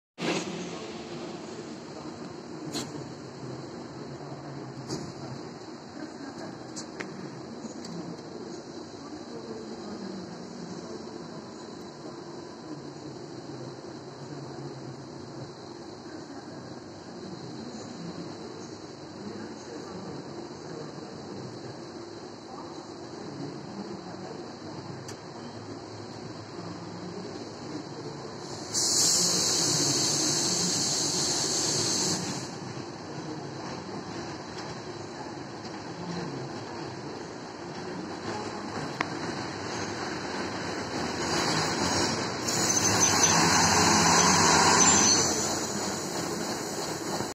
Тепловоз ЧМЭ3-5610 выполняет манёвр, ст. Адлер.
Видео снято на станции Адлер, Северо-Кавказская железная дорога.
Тепловоз ЧМЭ3-5610 построен в 1987 г. Приписан к ТЧЭ-16 Туапсе, Северо-Кавказская железная дорога.